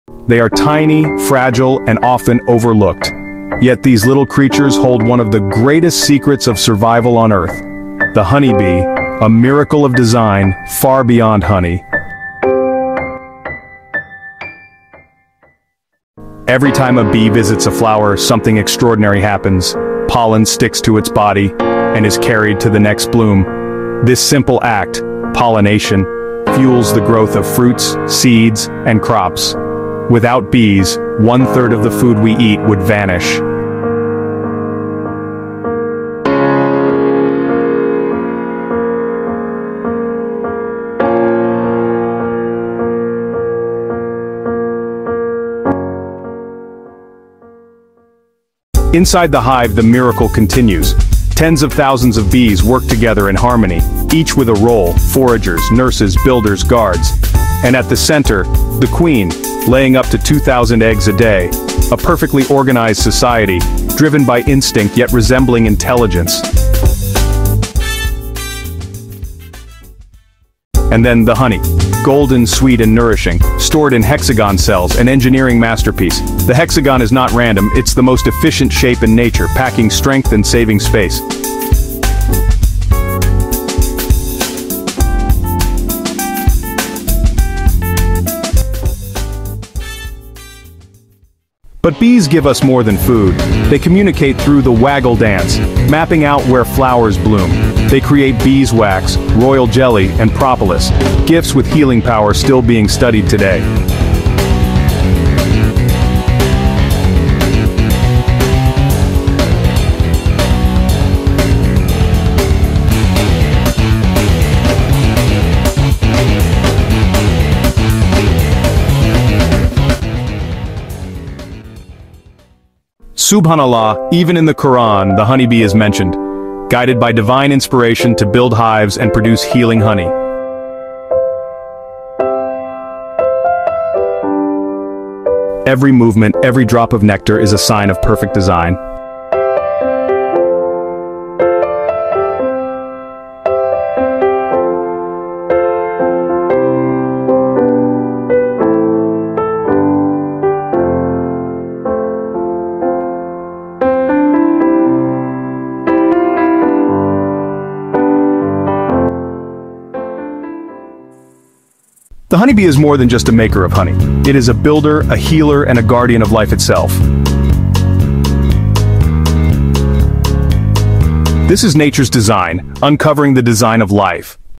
The Miracle Of The Honeybee – More Than Just Honey!
Discover the hidden world of honeybees in this short, cinematic exploration of pollination, hive life, and nature’s genius. We reveal how bees pollinate crops, communicate with the waggle dance, build hexagonal combs, and produce honey, beeswax, royal jelly, and propolis. Perfect for nature lovers and students, this video blends science, spirituality, and stunning visuals.